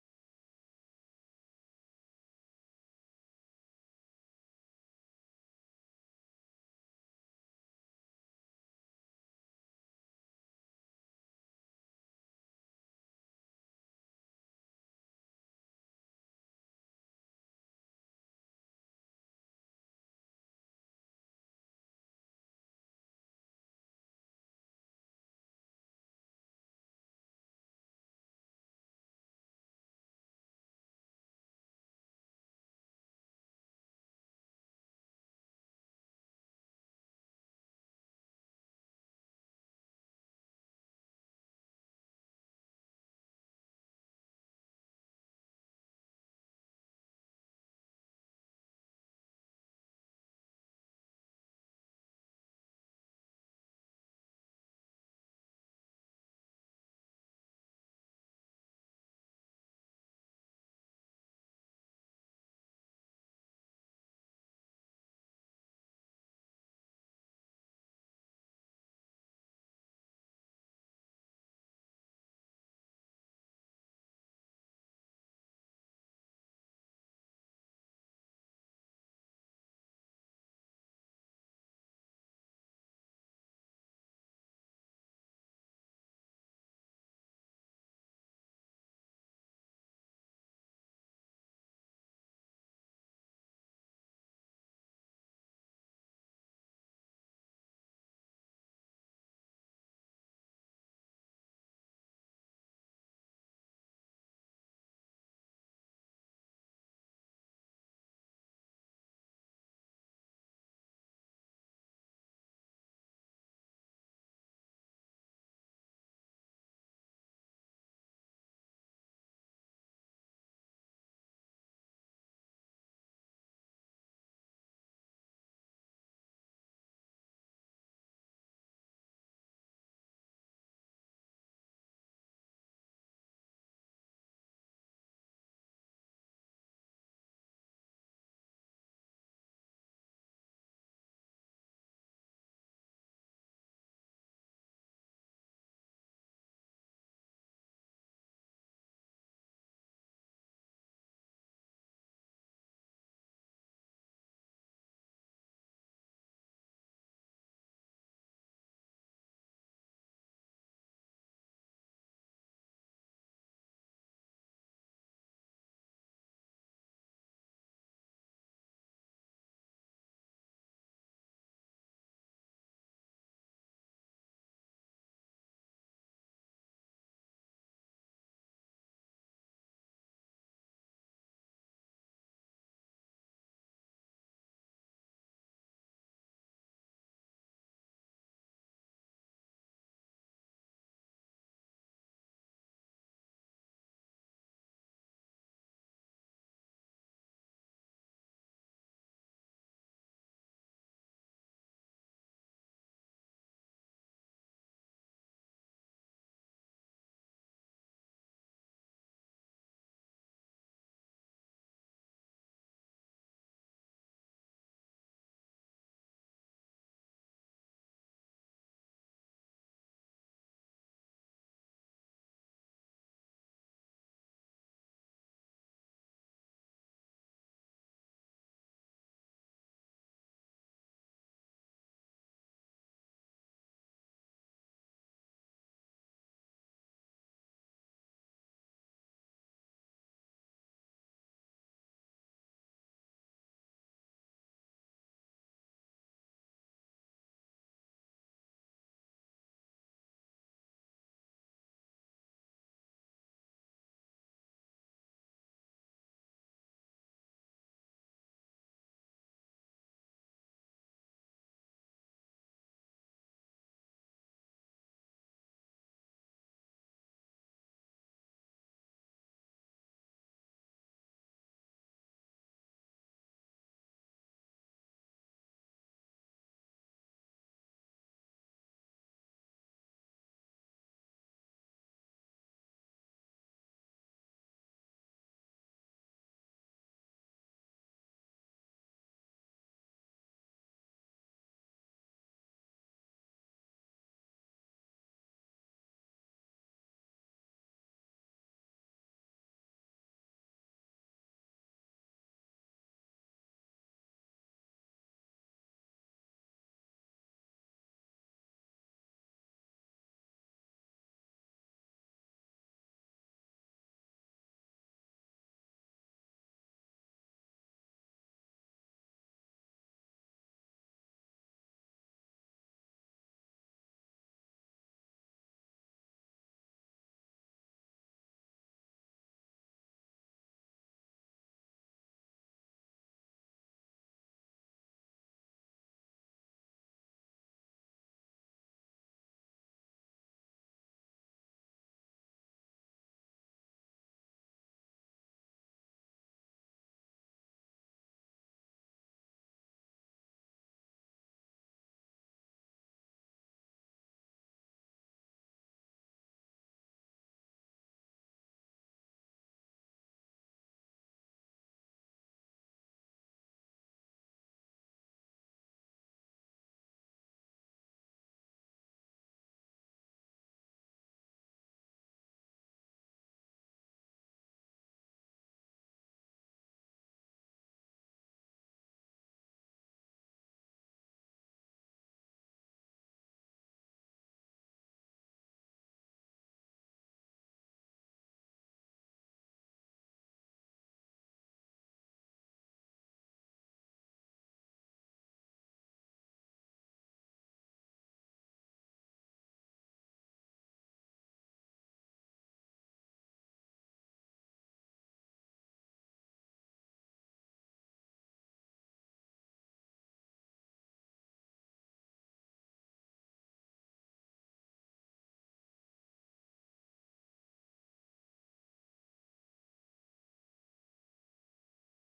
Racing A Level 133 Player! (INSANE)
The First Race Was My Fastest Time I Have Ever Done!